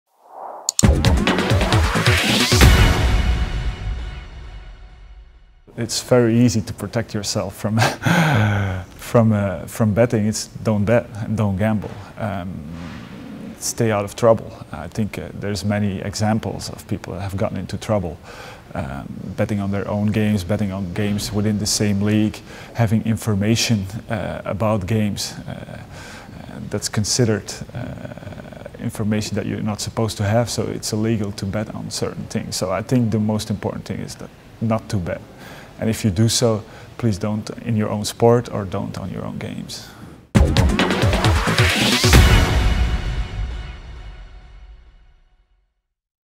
Tomas Van den Spiegel - How can I protect myself from illegal betting and match-fixing?
Tomas Van den Spiegel gives players tips to avoid legal trouble via illegal betting and match-fixing.